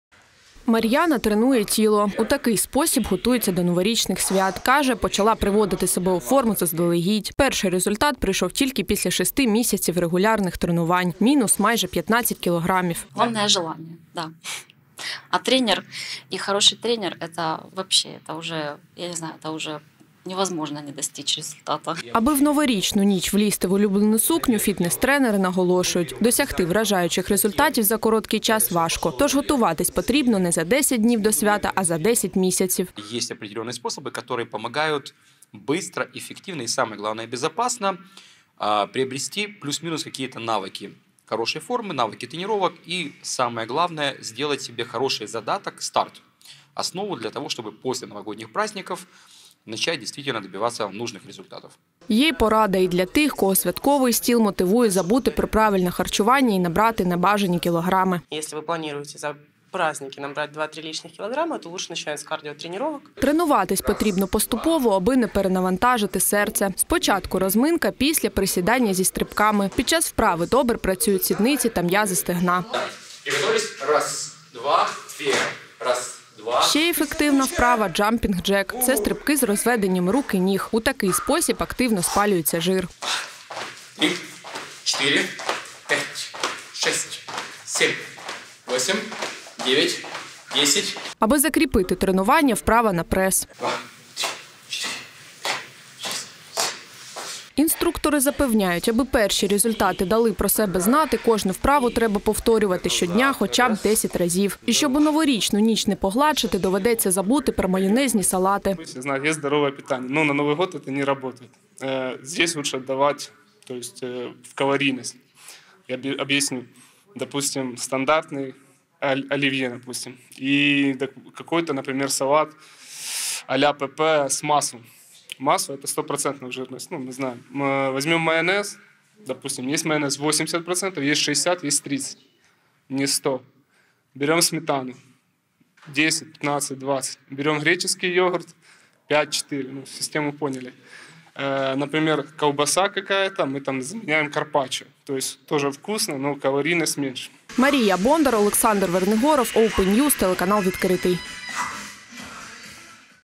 Поради від тренерів та дієтолога: як схуднути до Нового року
Свята, що наближаються, ще один привід зайнятися власною фігурою. Адже в новорічну ніч не хочеться думати про зайві кілограми. Як себе привести себе у форму за короткий термін – лайфхак від нашої Марії Бондар.